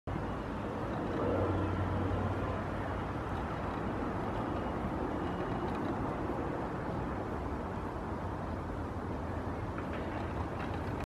Feral Rabbit grazing on the lawn
Feral European Rabbit (Oryctolagus cuniculus) grazing on the lawn.

Kaniini (Oryctolagus cuniculus) laiduntaa nurmikolla.